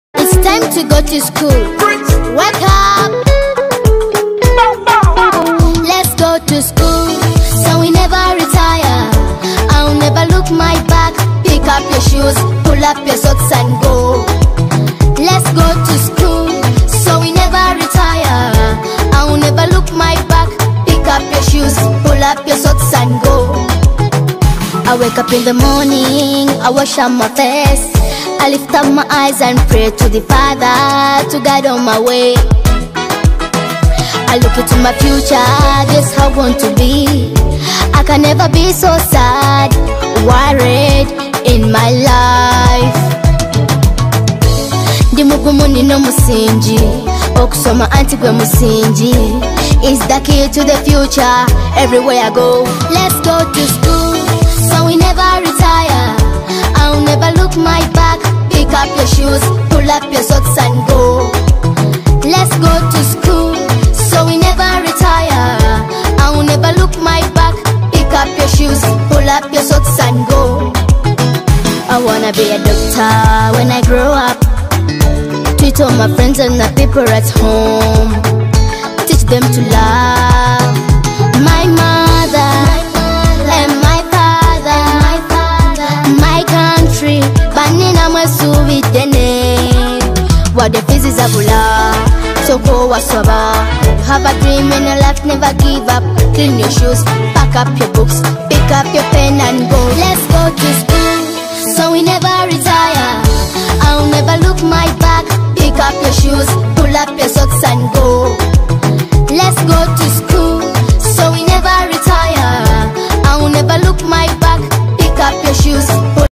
Let's go to school// Continental English Boarding Secondary School, Gaindakot-2 // New Admission
C.E.B.S.S was established in 2046 B.S (1989 A.D) by Mr. Rudra Kumar Pradhan with the aim of providing exceptional education while at the same time impacting well-rounded learning to students. Our motto is "Education with vision".